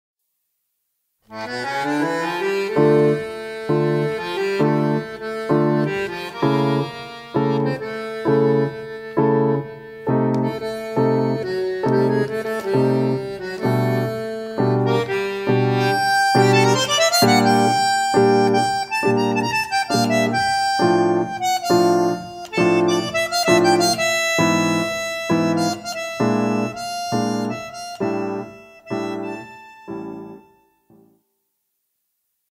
TRACK 57 - EL PIANO EN EL TANGO
Bandoneón: Eva Wolff

Material en audio y/o video perteneciente al libro "El Piano en el Tango" (Colección Método de Tango)

Audio - video file belonging of "The Piano in Tango" book (Metodo de Tango Collection)